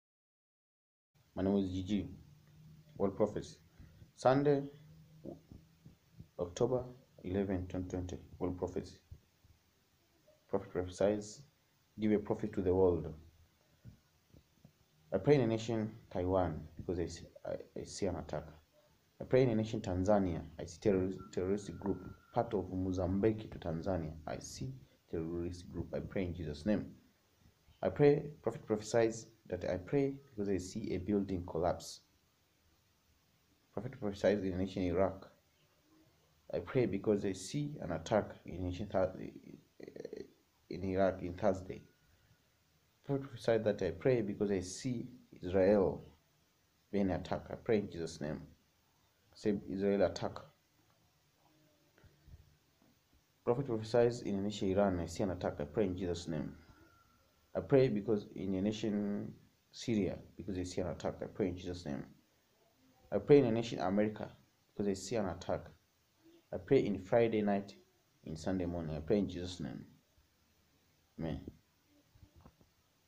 October 11, 2020 Sunday Oct 11, 2020 of World Prophecy
Sunday World Prophecy Oct 11, 2020
World Prophecy
1. I praying in a Nation Taiwan because i see an attack Prophecy.

2. Tanzania Praying for Terrors Group Attack party of Mozambique to Tanzania i see a Terrorist Group and i praying in Jesus Name.

3.Prophet Prophecies that i praying because i see buildings Collops.

4.Prophecies in nation Iraq i praying because i see an attack in Thursday.

5.Prophet Prophecies that i praying in a Nation Israel because i see an attack.

6. Prophet Prophecies in a Nation Israel i see an attack in Saturday i praying in Jesus Name.

7.I praying in a Nation Iran i see an attack and i praying in Jesus Christ.

8.I praying in a Nation Syria Because i see an attack and i praying in Jesus Name.

9.I praying in nation America because i see an attack i praying in Friday Night and Saturday Morning i praying in Jesus Name.